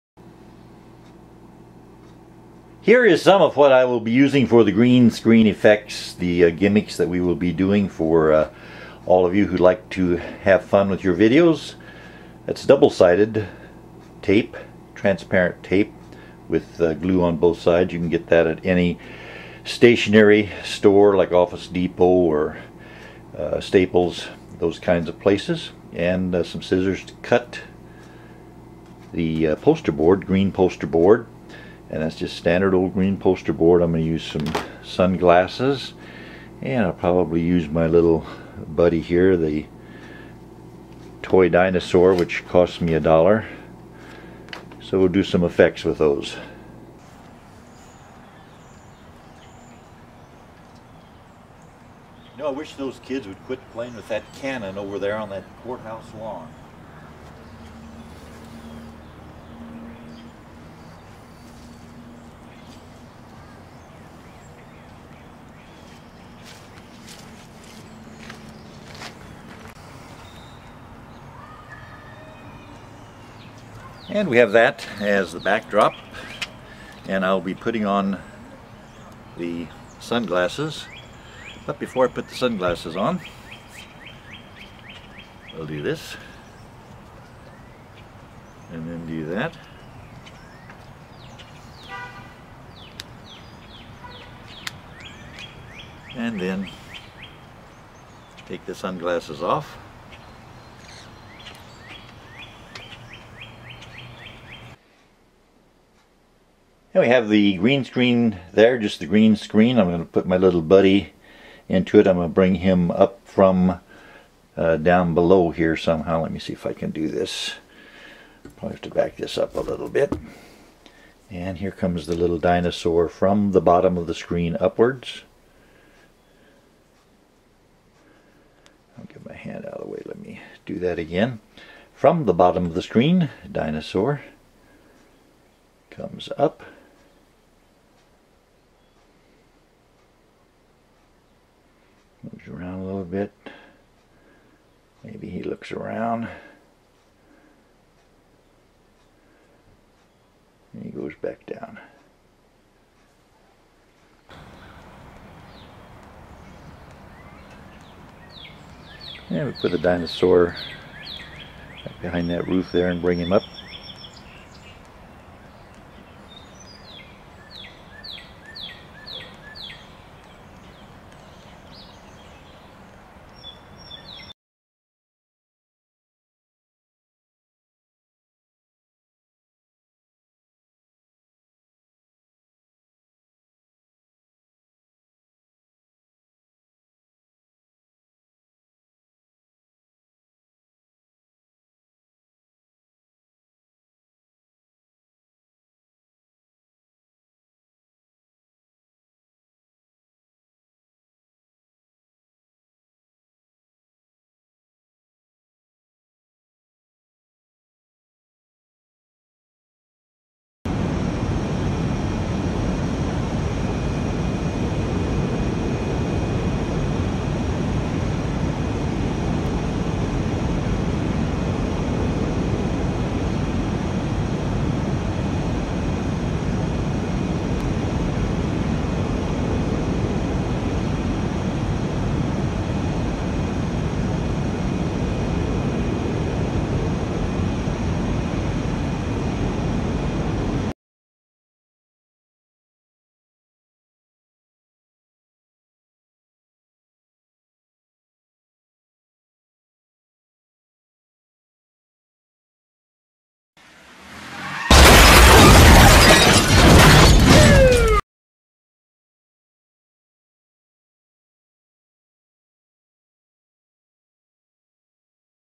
Green Screen Fun Effects Tutorial Tom Willett
Each video software that has Chroma Key or Green Screen capabilities will be able to do these effects. This is not the actor standing in front of a green screen. This is having the green screen as the top or overlay video. Even the dinosaur that appears to be behind a house is actually in the foreground and the house is the background. Use your imagination and some of the stock footage green screen videos at YouTube to create your own video scenes.